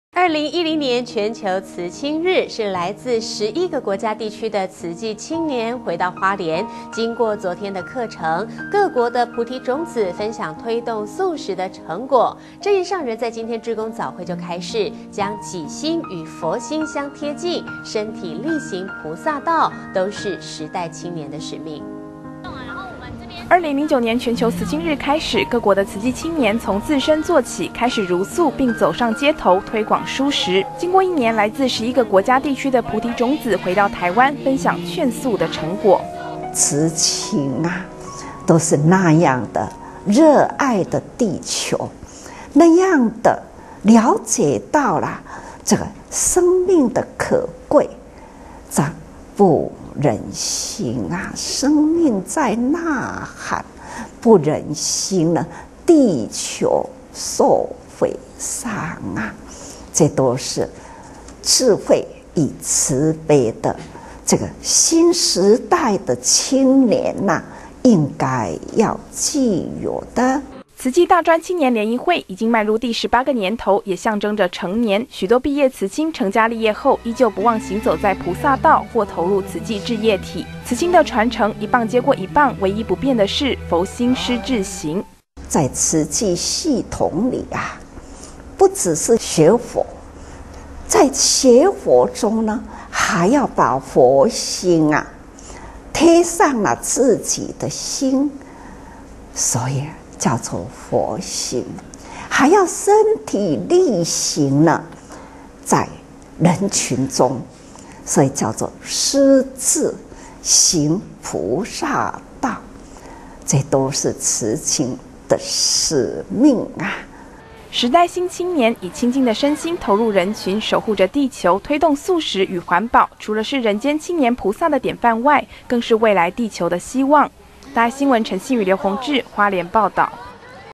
2010-12-26 上人開示 慈青善種蔚成林 秉持佛心志不移
2010/12/26上人開示 慈青善種蔚成林 秉持佛心志不移
2010年全球慈青日，來自11個家地區的慈濟青年回到花蓮，經過昨天(12/25)的課程，各國菩提種子分享推動素食的成果。證嚴上人在今天志工早會開示，將己心與佛心相貼近，身體力行菩薩道，這都是時代青年的使命。

2009年全球慈青日開始，各國的慈濟青年從自身做起，開始茹素，並走上街頭推廣蔬食。經過一年，來自十一個國家、地區的菩提種子回到台灣，分享「勸素」的成果。

證嚴上人開示：「慈青都是那樣的熱愛著地球，那樣的了解到生命的可貴，不忍心生命在吶喊，不忍心地球受毀傷，這都是智慧與慈悲的新時代的青年應該要具有的。」

「慈濟大專青年聯誼會」已經邁入第18個年頭，也象徵著「成年」，許多畢業慈青成家立業後，依舊不忘行走在菩薩道，或投入慈濟志業體。慈青的傳承，一棒接過一棒，唯一不變的是，佛心師志行。

證嚴上人開示：「在慈濟系統裡，不只是學佛，在學佛中，還要把佛心貼上了自己的心，所以叫做佛心，還要身體力行在人群中，所以叫做師志，行菩薩道，這都是慈青的使命。」

時代新青年，以清淨的身心投入人群，守護著地球，推動素食與環保，除了是人間青年菩薩的典範外，更是未來地球的希望。